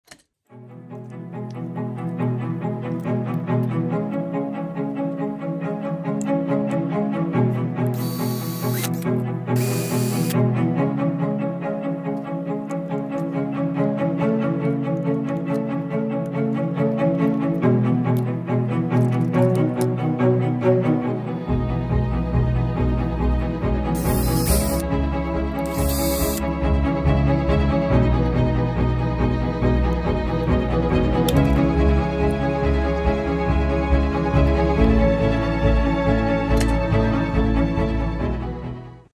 DIY Automatic Candle Extinguisher
Candle saver” device that automatically stops a candle from burning itself down DIY